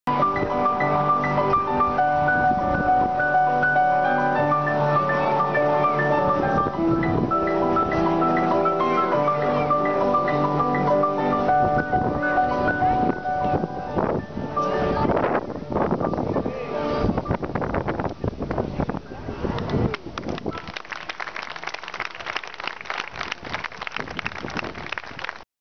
Illusionista parte 2
Alessio è un artista di strada. Guardate come gioca con queste sfere di metallo... sembrano un prolungamento della sua fantasia... San  Teodoro, Luglio 2014.